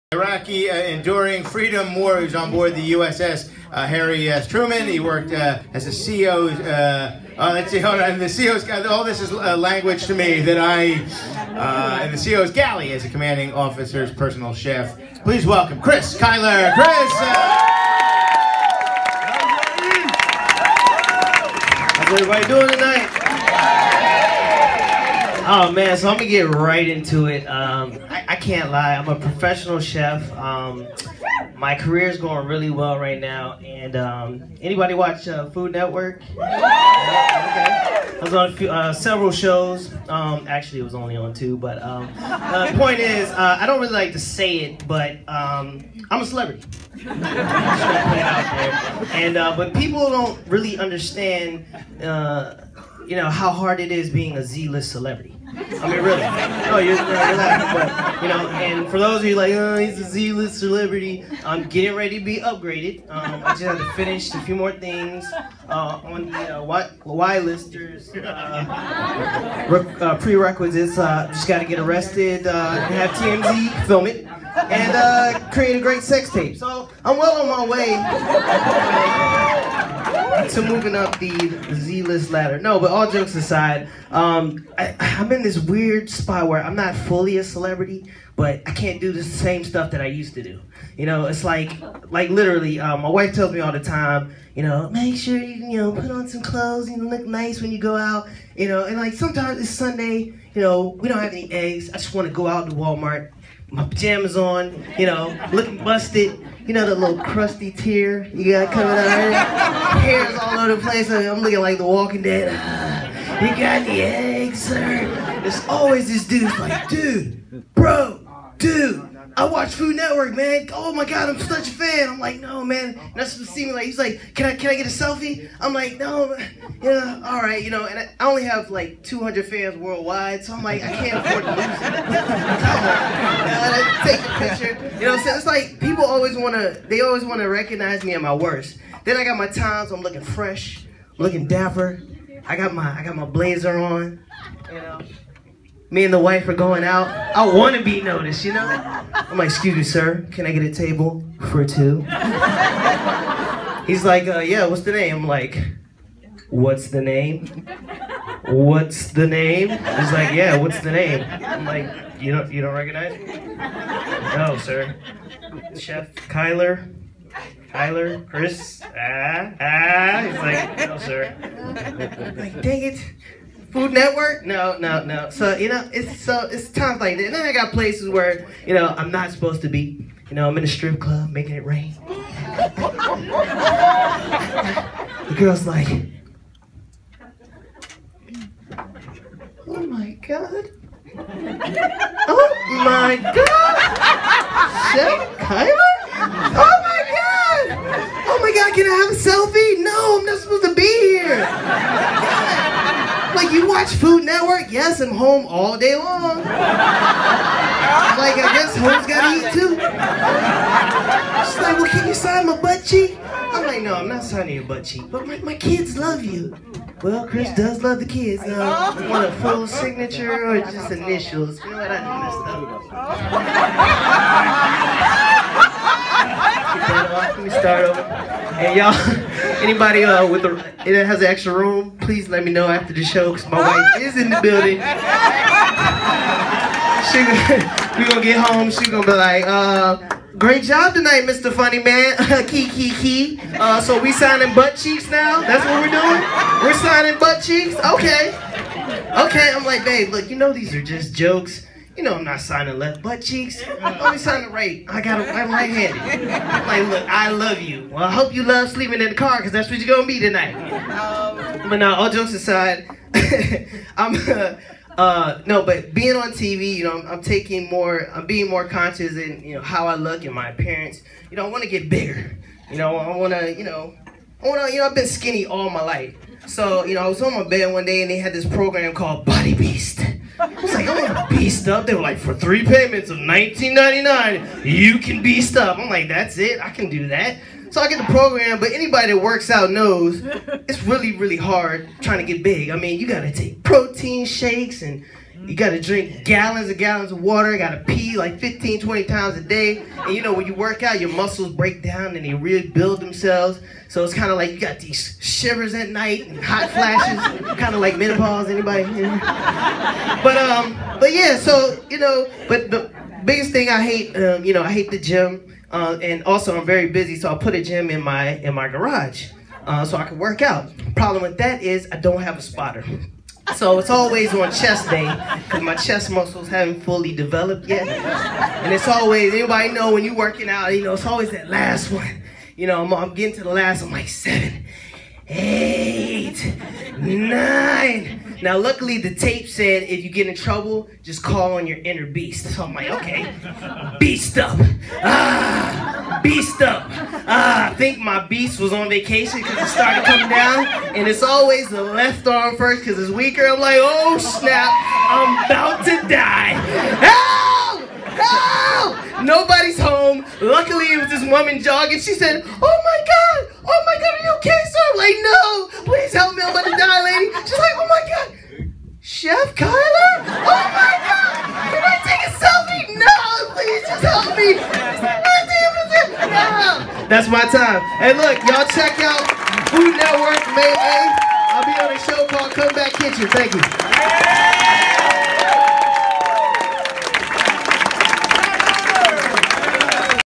5 mins to funny Comedy Bootcamp Graduation Show (Chris Kyler)
Chef Kyler's takes a stab at Stand-up.